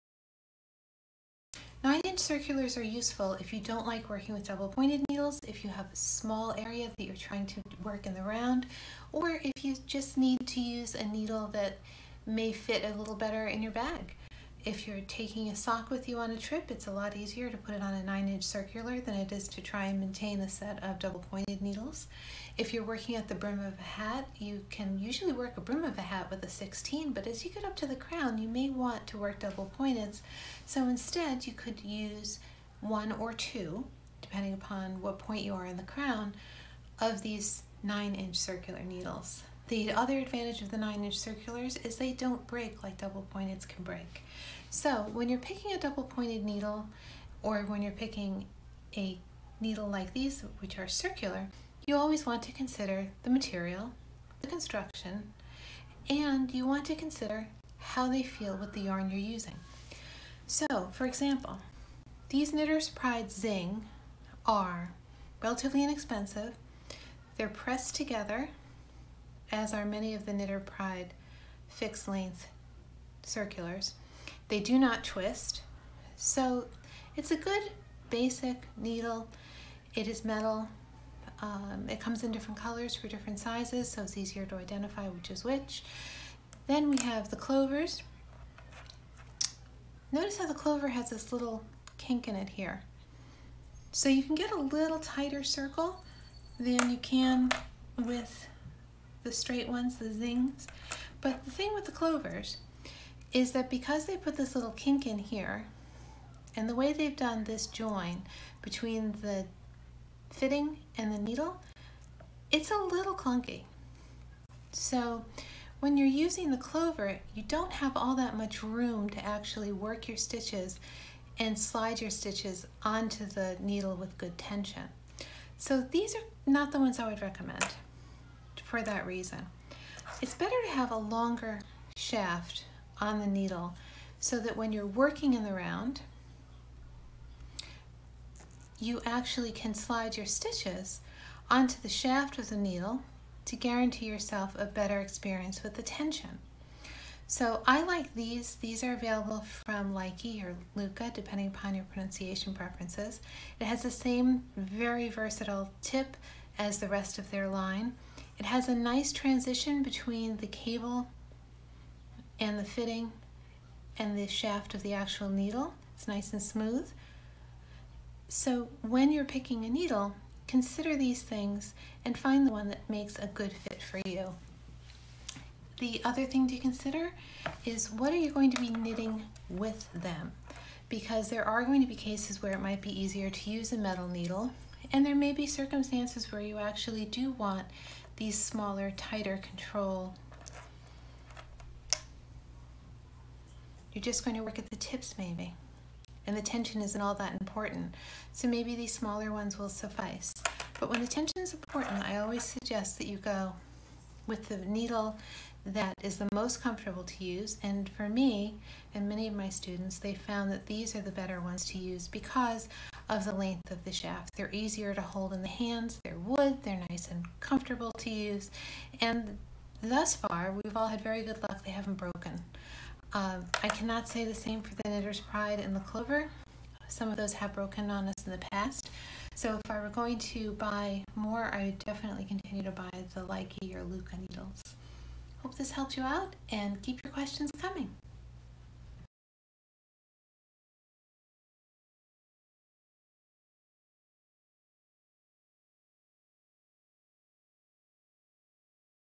Short circular needles 8" - 9"
A short discussion about short needles. Whether you choose metal or wood, the options are not limited to a particular brand nor style but they are very hard to find on line and in stores so when you find what you love, you'll want to buy more than one!

Things I have made with 9" circular needles:
Using two at a time in lieu of double pointed needles:
A hat

Using 1 9" circular
Mitts

Cozy Coastal Knits

Buy my book for more great tips and tricks, don't let the cover fool you, the first 37 pages are all technique related